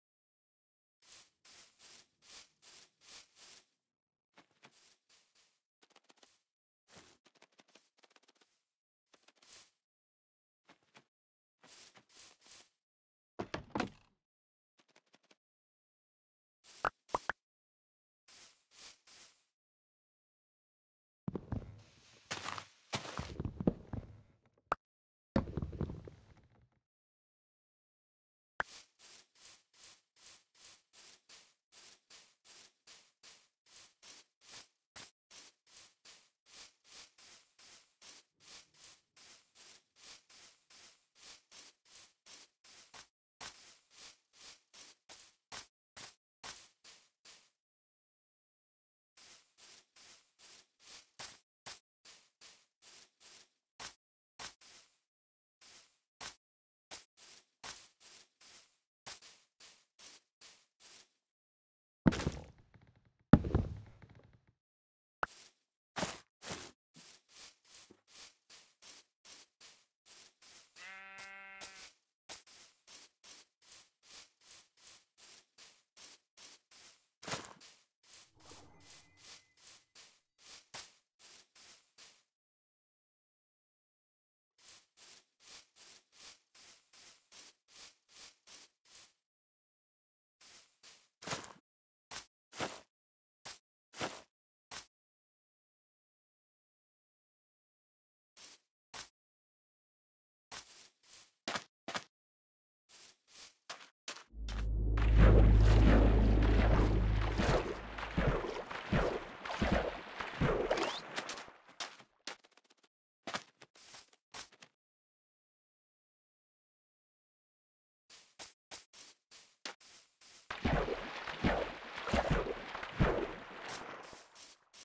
I found a jungle! (+ parrots, bamboo, cocoa beans and a tiger 🐅)
Beautiful biome. I wish I was living in a jungle.